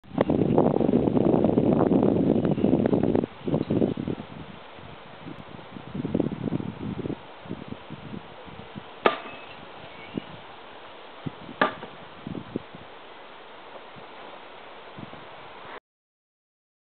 knife throwing-busting balloon from 2.5 spins
Throwing knife 2.5 spins and busting balloon. Balloon is attached to a leaning post.Throw is made from 23 feet away.